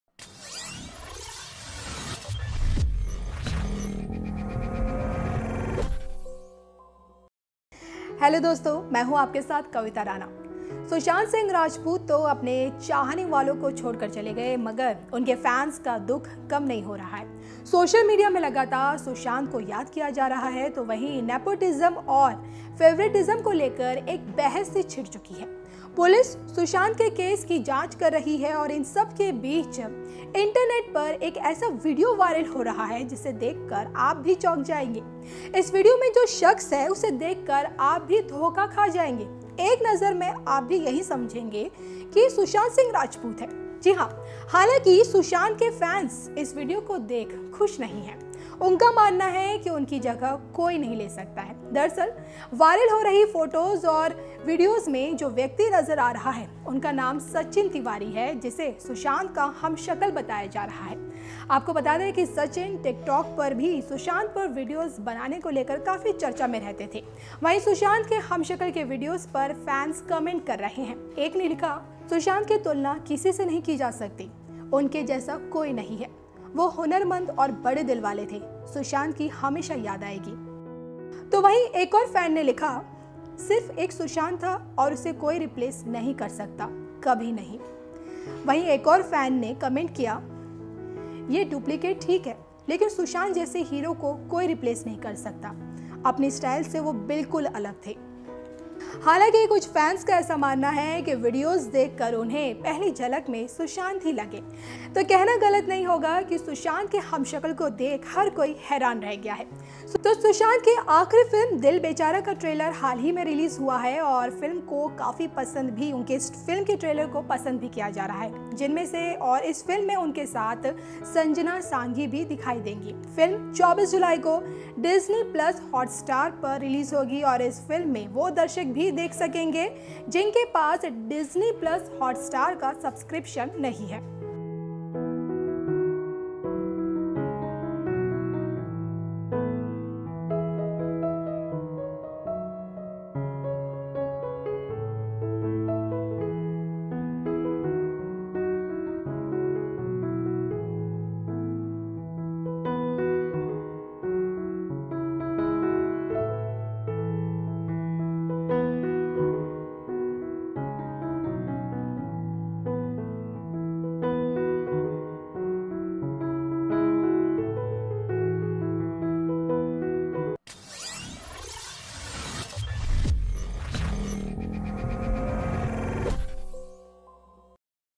Sushant के हमशक्ल को देख नाराज हुए Fans, बोले- उसे कोई रिप्लेस नहीं कर सकता
सुशांत सिंह राजपूत तो आपने चाहने वालों को छोड़कर चले गए, मगर उनके फैंस का दुख कम नहीं हो रहा... सोशल मीडिया में लगातार सुशांत को याद किया जा रहा है... वहीं, नेपोटिज्म और फेवरेटिज्म को लेकर एक बहस भी छिड़ी हुई है.. पुलिस सुशांत के केस की जांच कर रही है.. इन सबके बीच इंटरनेट पर एक ऐसा वीडियो वायरल हो रहा है, जिसे देखकर आप भी चौंक जाएंगे..

Edited By - Shrestha Singh
Producer- Prashant